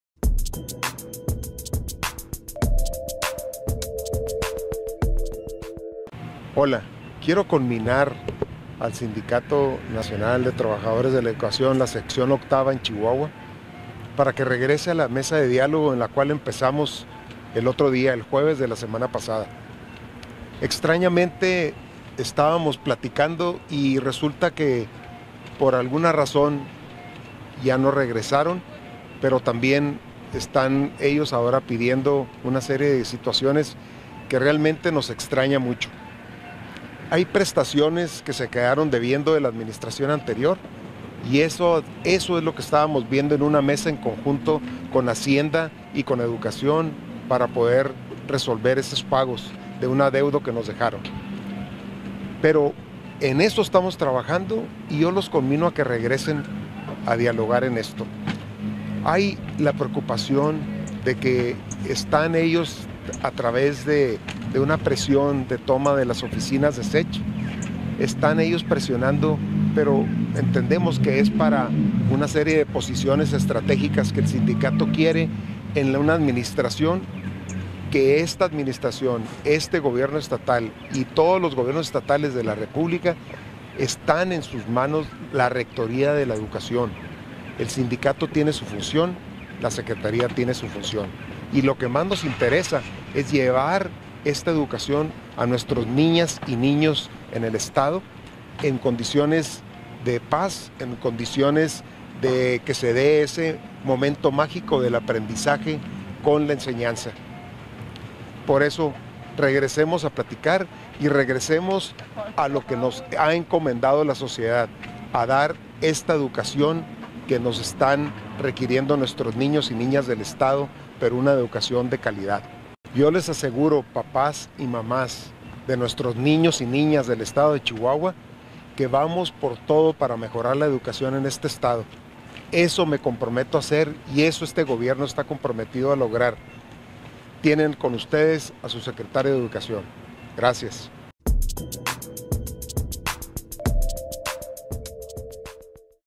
Pide secretario de Educación que la SNTE regrese al diálogo en Chihuahua
El secretario de Educación Cultura y Deporte de Chihuahua, Pablo Cuarón Galindo, pidió a los maestros cordura y diálogo para resolver por la vía pacífica estas demandas, luego del anuncio de paro de labores de este miércoles en toda la entidad.